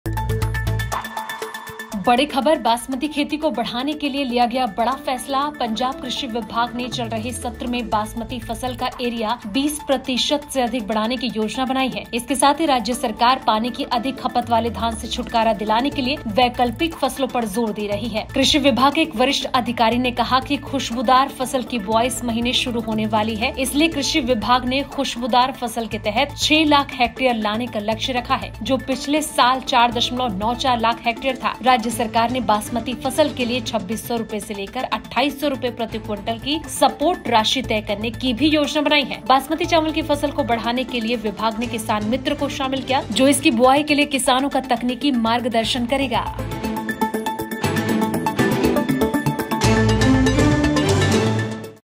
Basmati in Punjab: बासमती खेती को मिलेगा बढ़ावा, किसानों को किया जाएगा गाइड | Kisan Tak
बासमती खेती को बढ़ाने के लिए लिया गया बड़ा फैसला. पंजाब कृषि विभाग ने चल रहे सत्र में बासमती फसल का एरिया 20 प्रतिशत से अधिक बढ़ाने की योजना बनाई है .इसके साथ ही, राज्य सरकार पानी की अधिक खपत वाले धान से छुटकारा दिलाने के लिए वैकल्पिक फसलों पर जोर दे रही है. कृषि विभाग के एक वरिष्ठ अधिकारी ने कहा कि चूंकि खुशबूदार फसल (Aromatic Crop) की बुआई इस महीने शुरू होने वाली है, इसलिए कृषि विभाग ने खुशबूदार फसल के तहत छह लाख हेक्टेयर लाने का लक्ष्य रखा है, जो पिछले साल 4.94 लाख हेक्टेयर था. राज्य सरकार ने बासमती फसल के लिए 2,600 रुपये से 2,800 रुपये प्रति क्विंटल की सपोर्ट राशि तय करने की भी योजना बनाई है. बासमती चावल की फसल को बढ़ावा देने के लिए विभाग ने 'किसान मित्र' को शामिल किया है, जो इसकी बुआई के लिए किसानों का तकनीकी मार्गदर्शन करेगा.